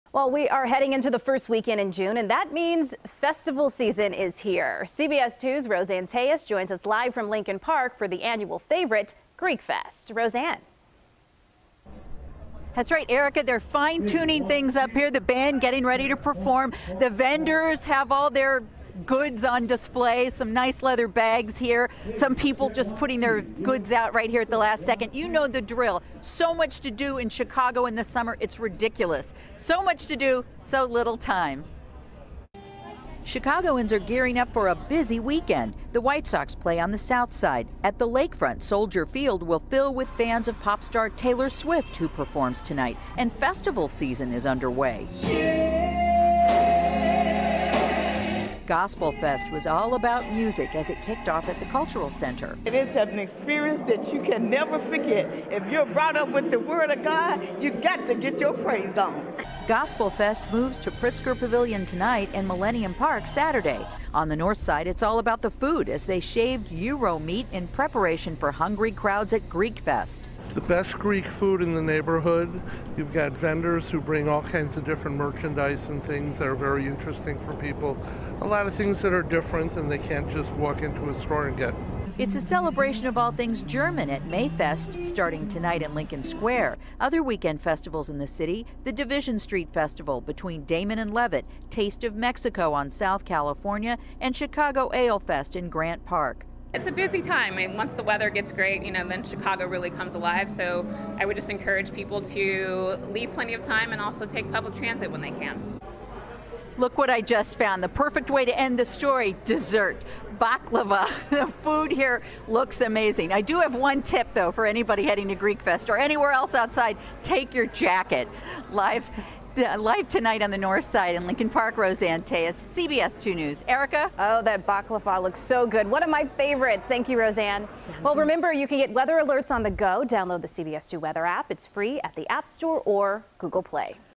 Festival Season Starts This Weekend
CBS 2's Roseanne Tellez reports on what the first weekend in June will bring to Chicago: the official start of festival season.